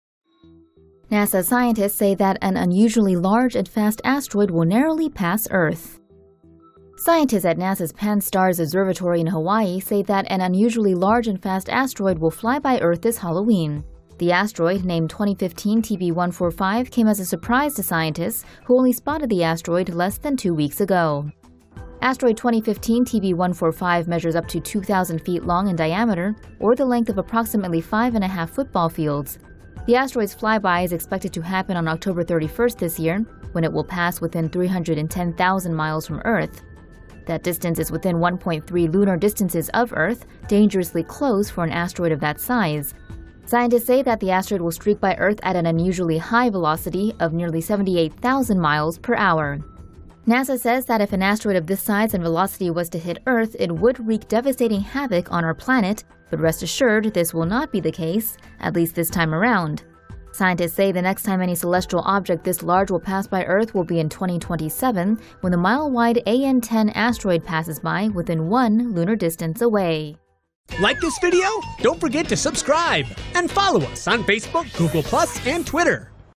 Halloween asteroid flyby will 'narrowly' miss Earth at the end of October - TomoNews
MAUI, HAWAII — Scientists at NASA's Pan-STARRS observatory in Hawaii say that an unusually large and fast asteroid will fly by Earth this Halloween. The asteroid, named 2015 TB145, came as a surprise to the scientists, who spotted it less than two weeks ago.

The asteroid's flyby is expected to happen on October 31st, when it will pass within 310,000 miles, or 1.3 lunar distances, from Earth at an "unusually" high velocity of nearly 78,000 miles per hour. 2015 TB145 has an "eccentric" and "high-inclination" orbit, which may explain its late discovery.

Estimated to be between 920 to 2,000 feet in diameter, the asteroid may seem far away enough, but actually it is flying by Earth dangerously close for an object of its size. According to NASA, if an asteroid of its size and velocity was to hit Earth, it would devastating our planet.

Scientists say the next time any celestial object this large will pass by this close to Earth will be in 2027 when the mile-wide AN10 asteroid passes within one lunar distance.